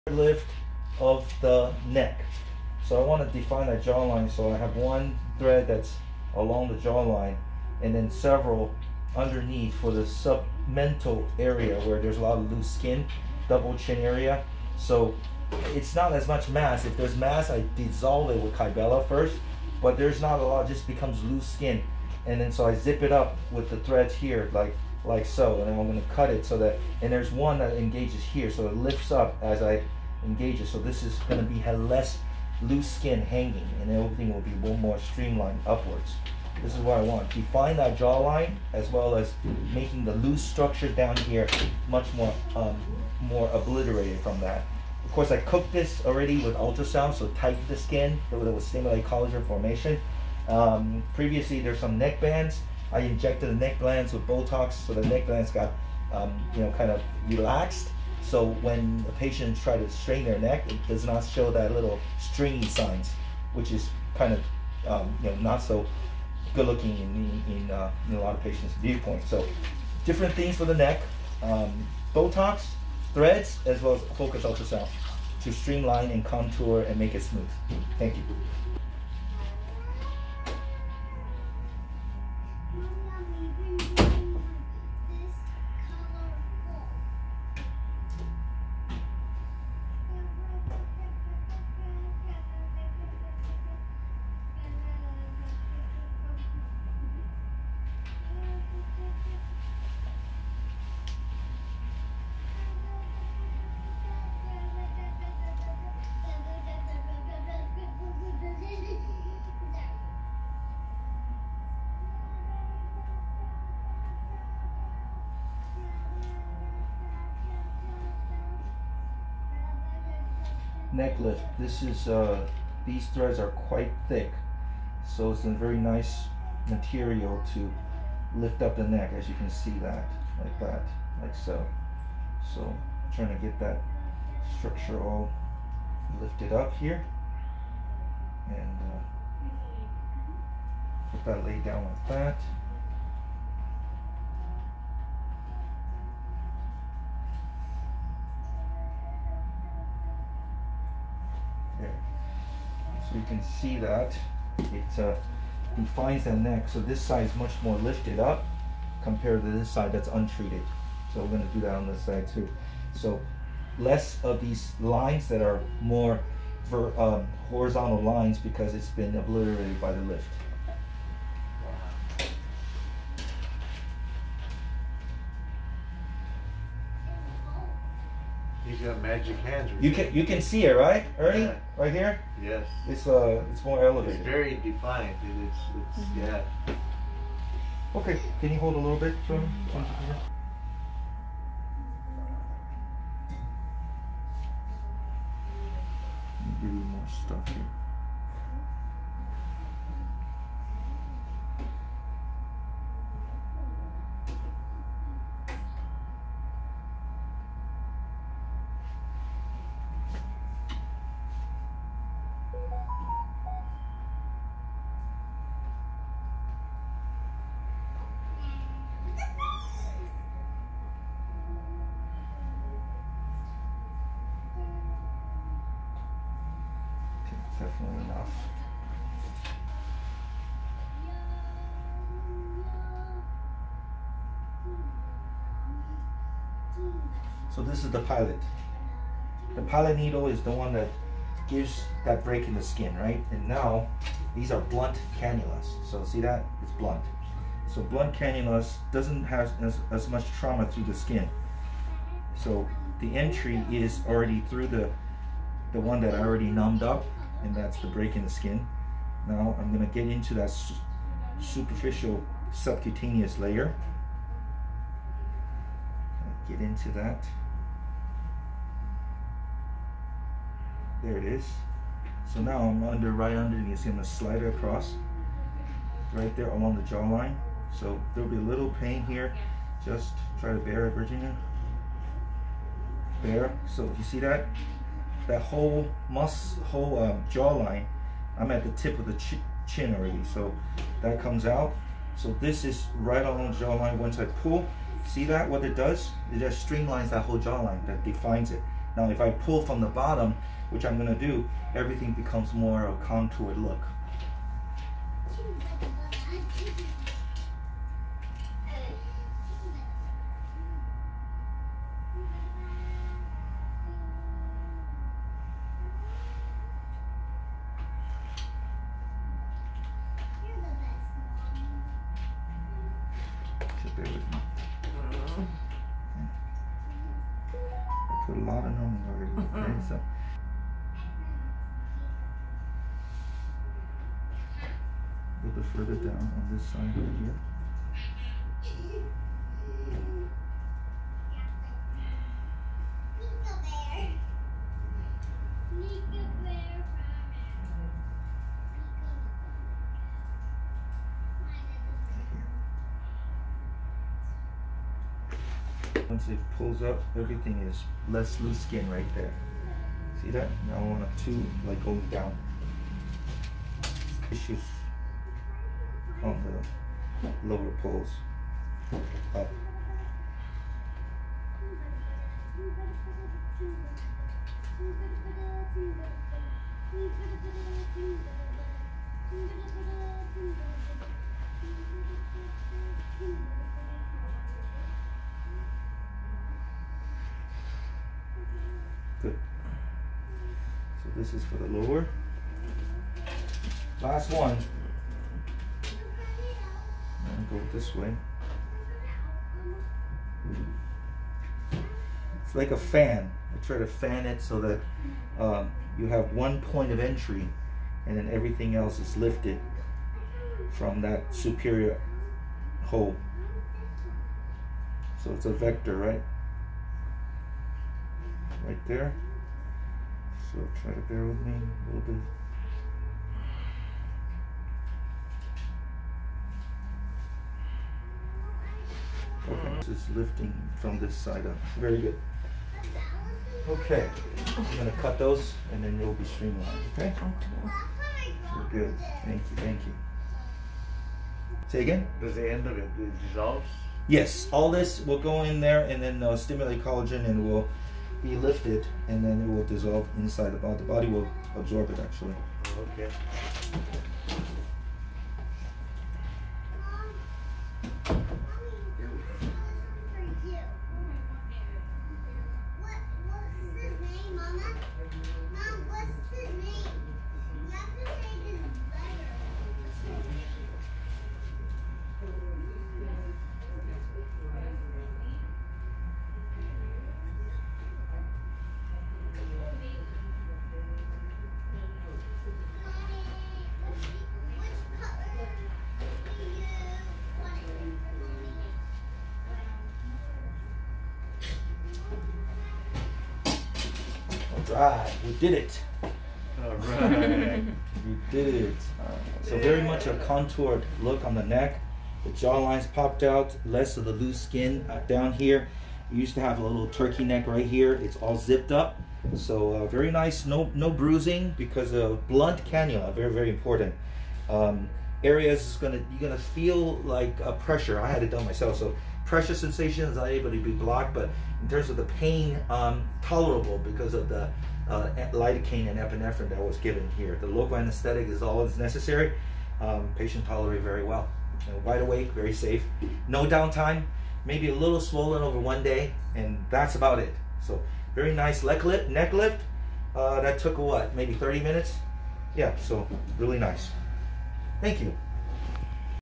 Renewing Your Youth And Beauty With A Face Lift
Using a combination of threads, ultrasound laser to tighten the skin and botox, patient's neck results in a defined, youthful and tightened neckline